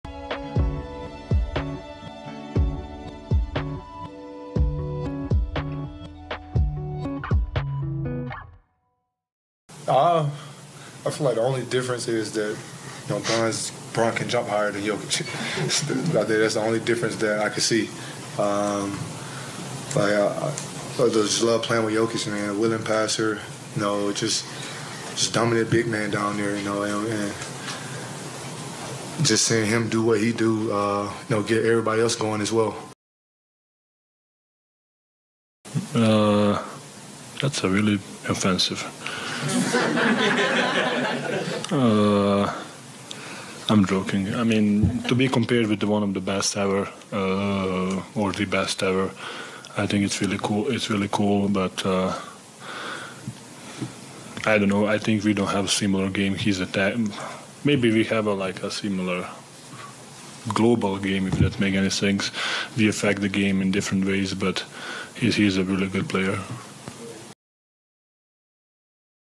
Jokic laughs off KCP's tongue-in-cheek comment that Lebron can jump higher than him｜Lakers｜Nuggets
Nikola Jokic has a chilled reaction to teammate Kentavious Caldwell-Pope's tongue-in-cheek claim that LeBron James jumps higher than the Nuggets megastar.
Caldwell-Pope played in the Lakers' 2020 championship team, which was led by James, and was asked what was the difference between those two.
Jokic is coming off a stellar Game 1, which saw him recorded his sixth triple-double of these playoffs with 34 points, 21 rebounds and 14 assists, powering the Nuggets to a 132-126 win over the Lakers in the opener of the Western Conference finals.